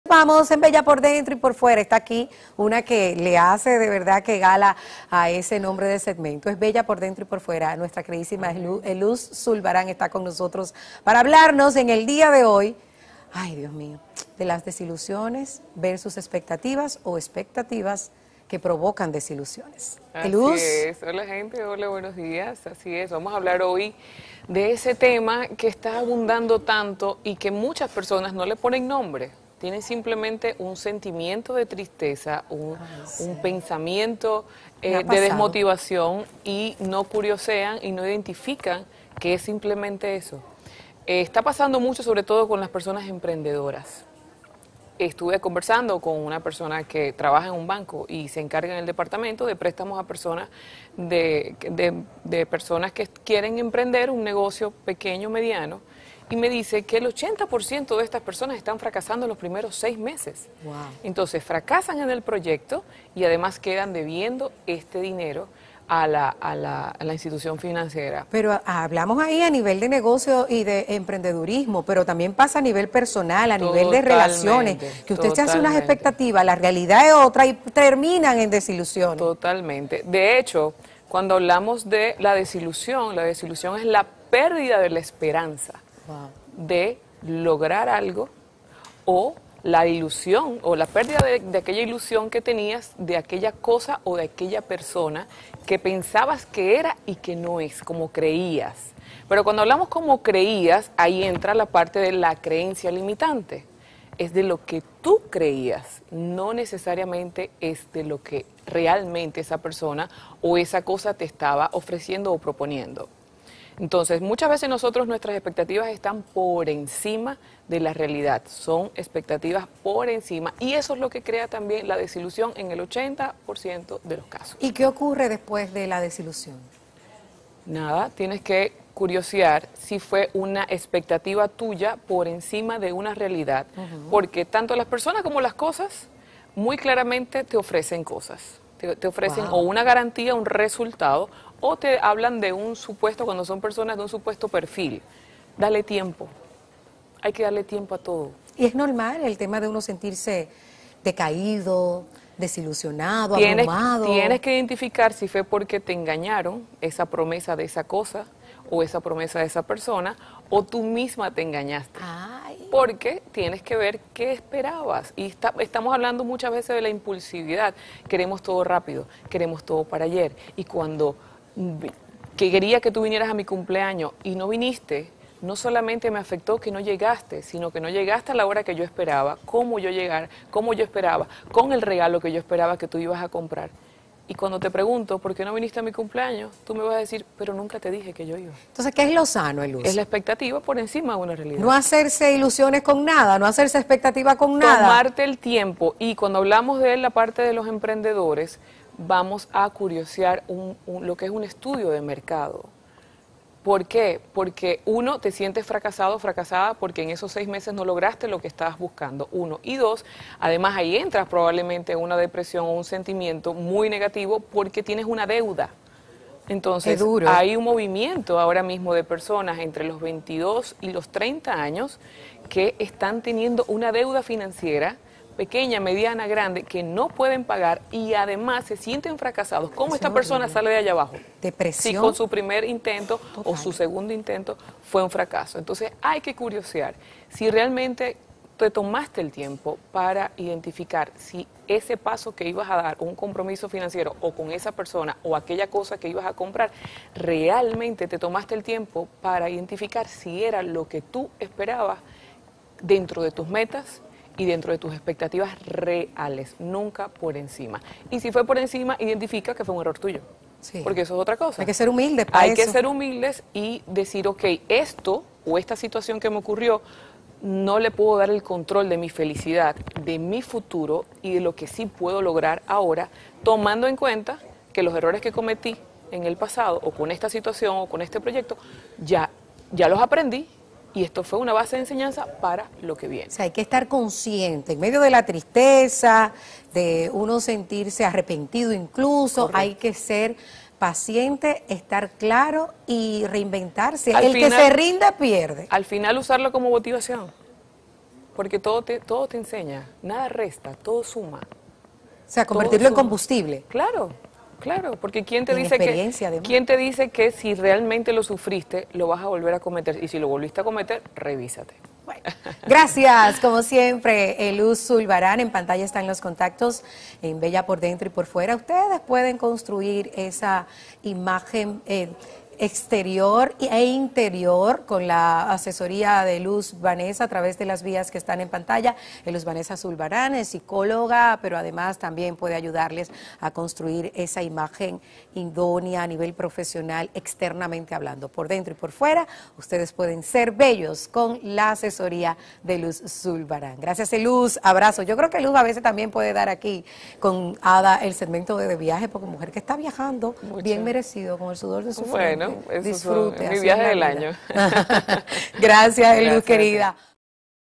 ¿Por qué las expectativas causan desilusiones?
La experta en belleza por dentro y por fuera Ellus Sulbarán, nos cuenta por qué las expectativas causan desilusiones.